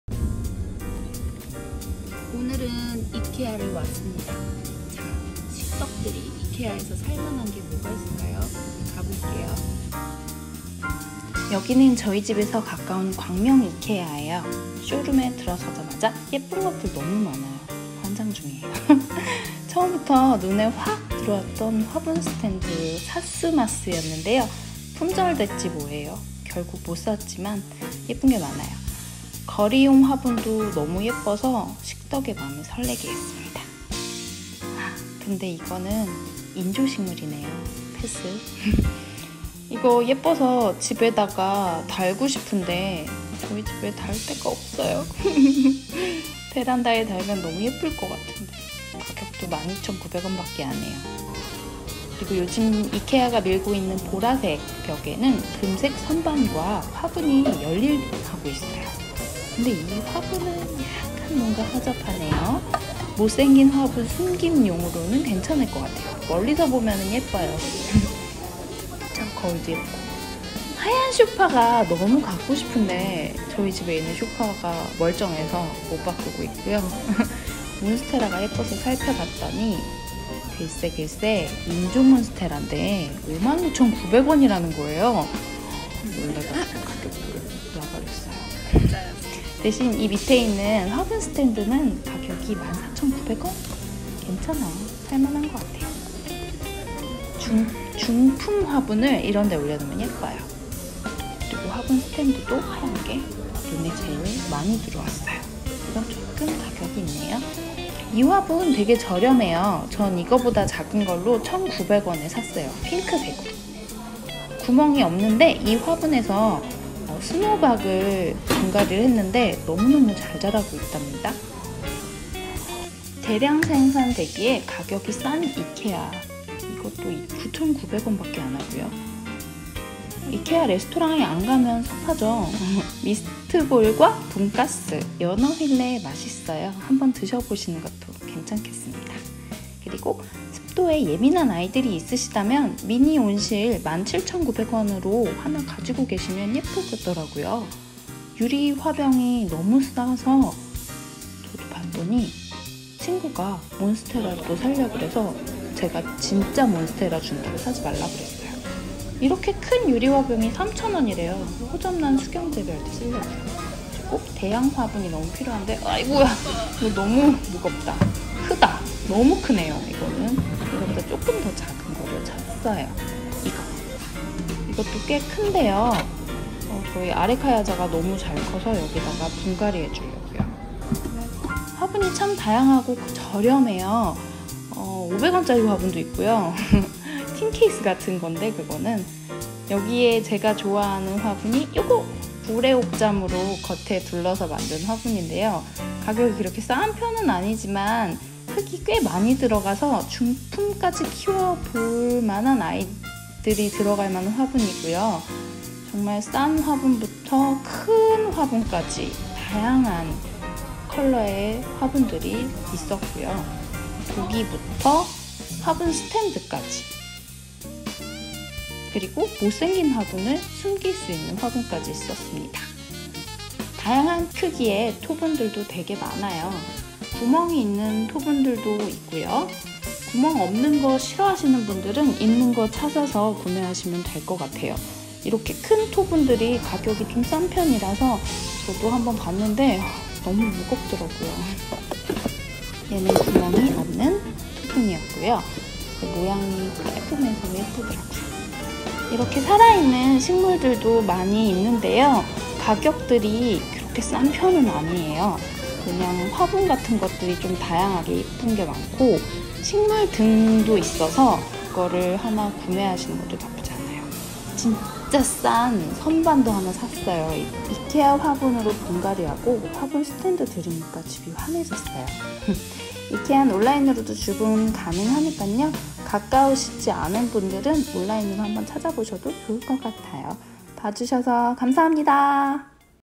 이케아에서 식덕 가드닝 아이템 쇼핑하기_화분, 화분선반과 식물등!
이케아에 식덕이 떴습니다! 식물을 사랑하는 사람들은 이케아에서 무엇을 사면 좋을까요? 이것만은 꼭 사야한다? 추천하는 화분들과 화분스탠드 가격과 실제 크기 보여드릴게요! 이케아 광명점에 함께 쇼핑하러 가요~!

구독자 551